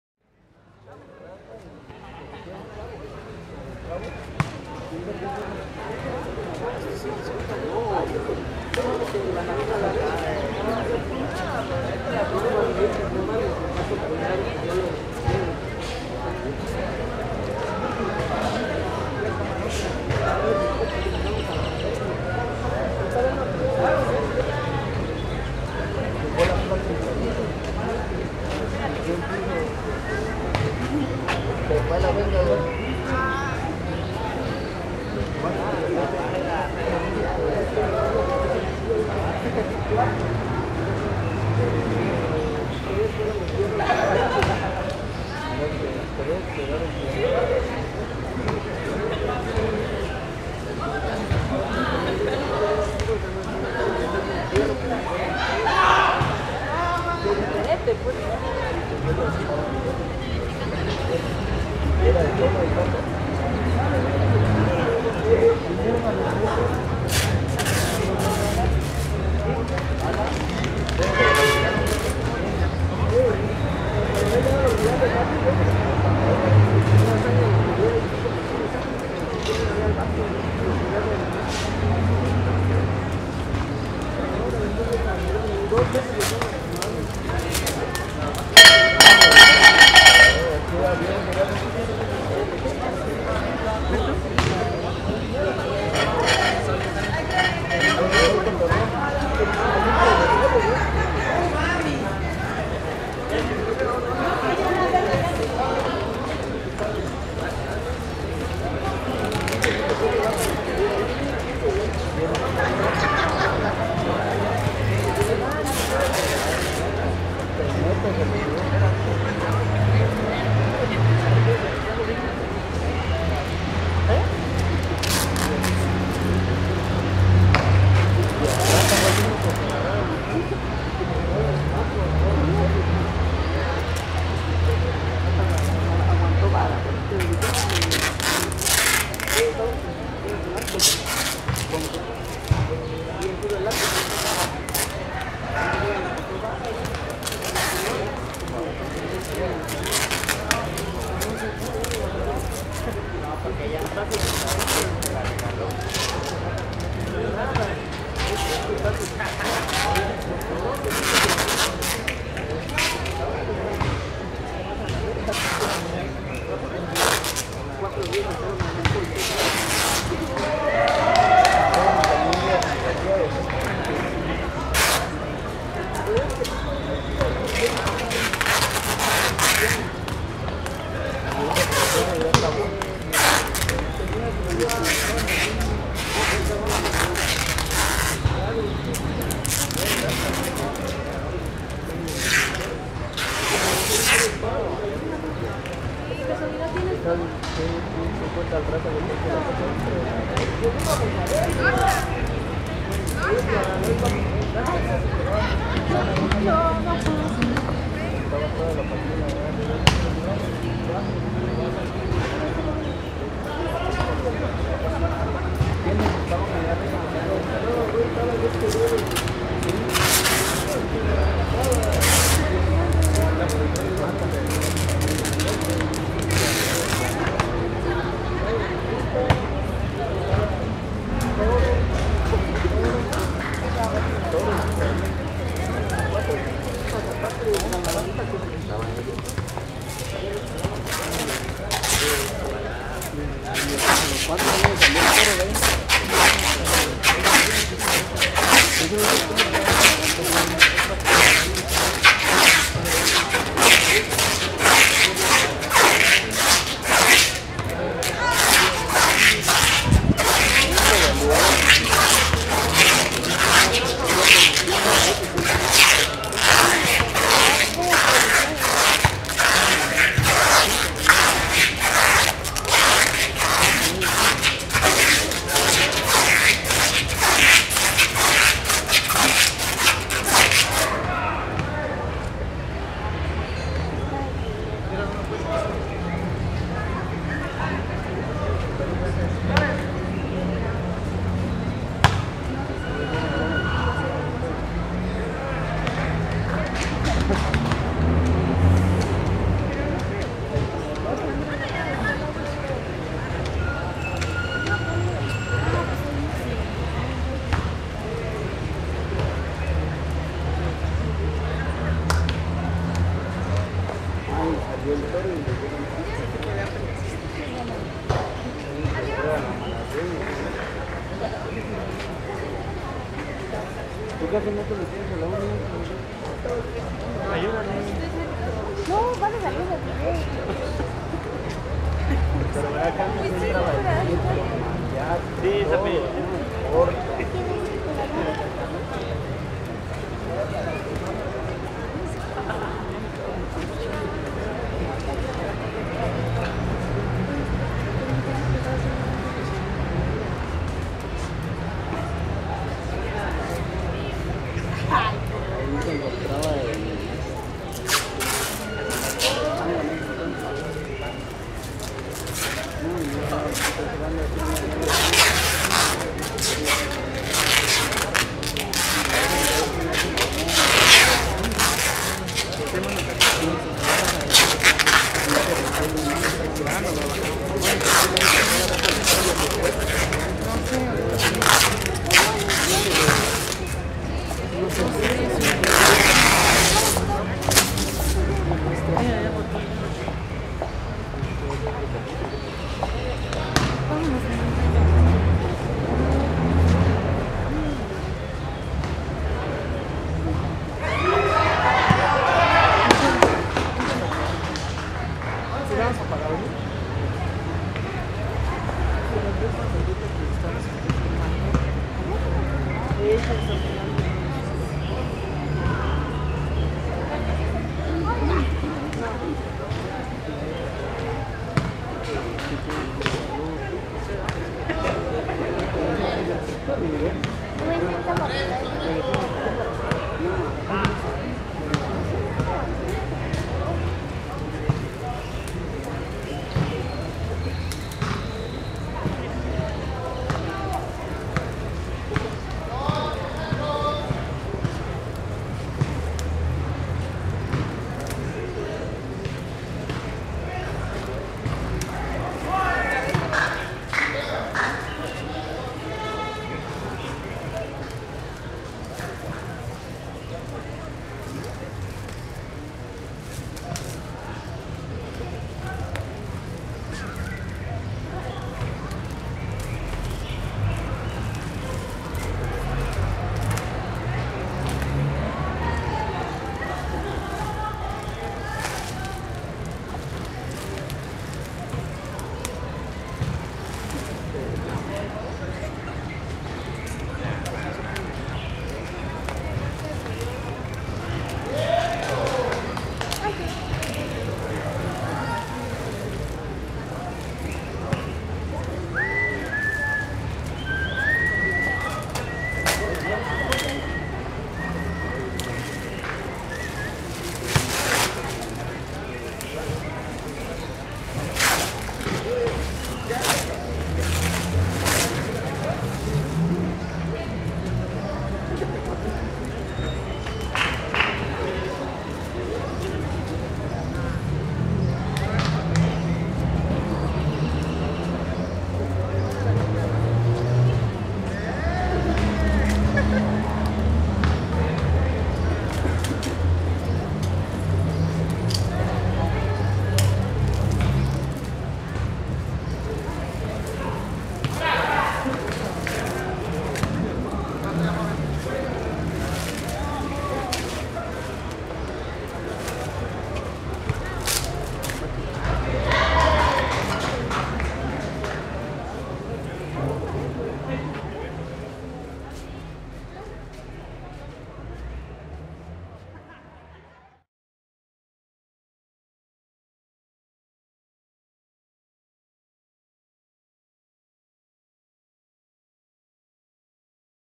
Acción 3 de Serie "Capullo", Oaxaca, México, Mayo 2016
Acción 3
Serie Capullo
Oaxaca, México
Festival Habemus Vivo
Curador Abdías Ramírez
Primer Encuentro Universitario de Exhibición y Taller Laboratorio de Performance Arte Acción
Mayo 2016